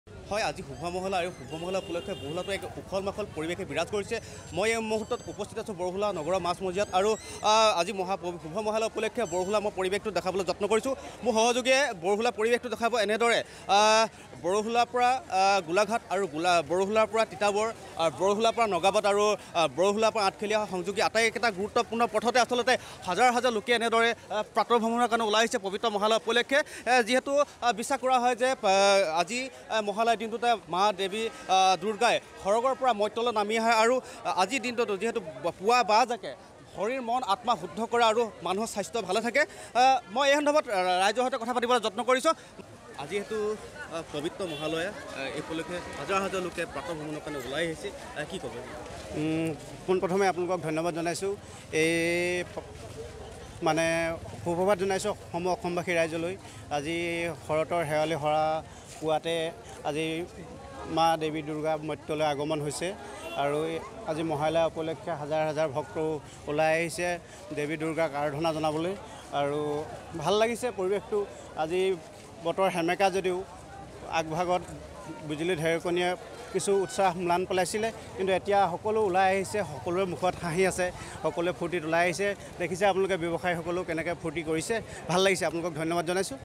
ৰাজ্যৰ বিভিন্ন প্ৰান্তৰ লগতে বৰহোলাতো উখলমাখল পৰিৱেশে বিৰাজ কৰিছে ৷
আজি মহালয়া,ৰাজ্যৰ বিভিন্ন প্ৰান্তৰ লগতে বৰহোলাতো উখলমাখল পৰিৱেশে বিৰাজ কৰিছে ৷ পুৱা প্ৰাতঃভ্ৰমণৰ কাৰণে হাজাৰ হাজাৰ লোক ওলাই আহিছে ৷ চাৰিওফালে বিৰ দি বাট নোপোৱা অৱস্থা ৷ আমাৰ এক প্ৰতিবেদন